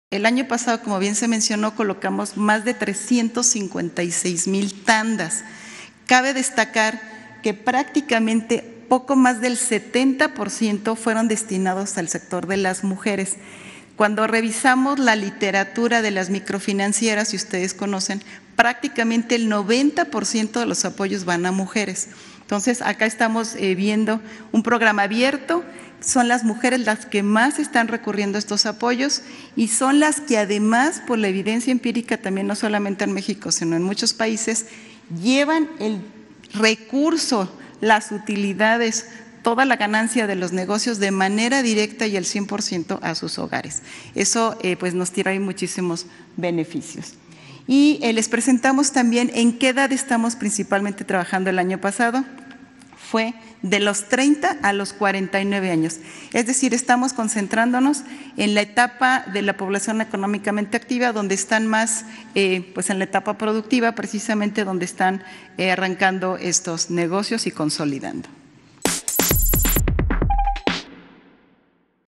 Más del 70% de tandas destinadas a mujeres
Rocío Mejía, titular del programa Microcréditos para el Bienestar, señaló que el año pasado se dieron 356 mil 540 créditos por un monto de 2 mil 139.2 millones  y que «prácticamente el 90% de los apoyos van a mujeres».